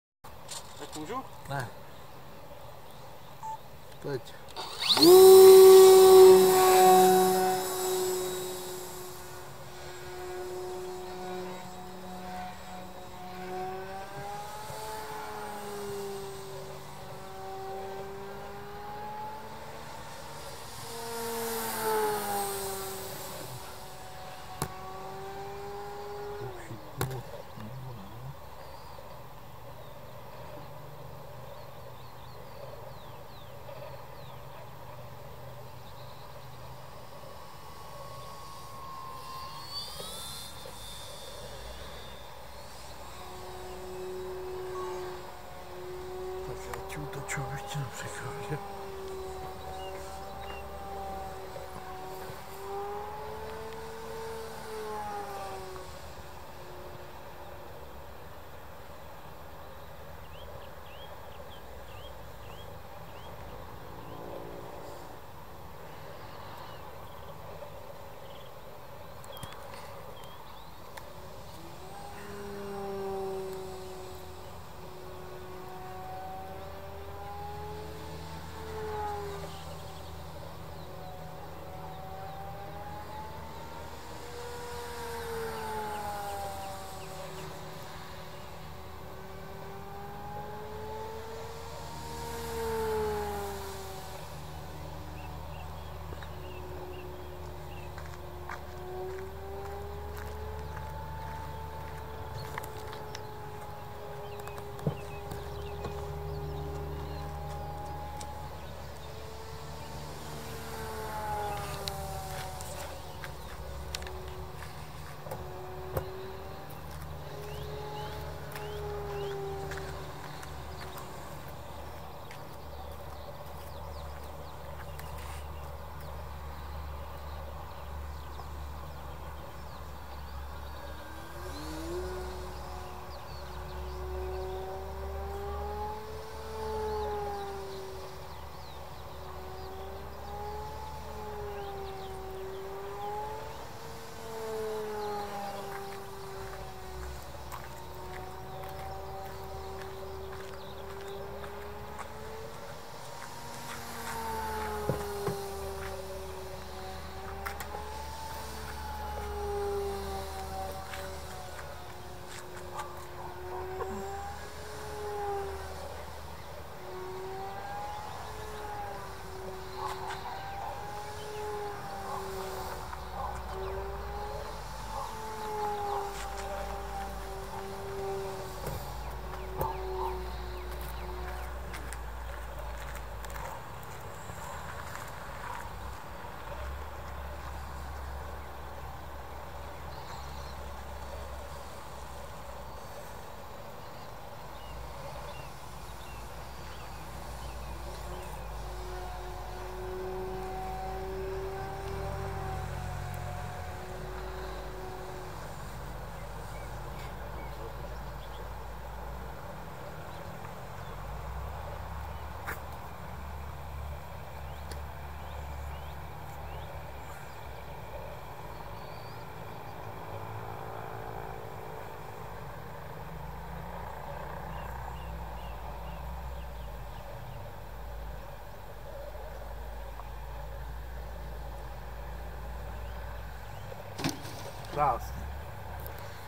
Rc model Pirát
Kvalitnější video z létání s modelem Pirát.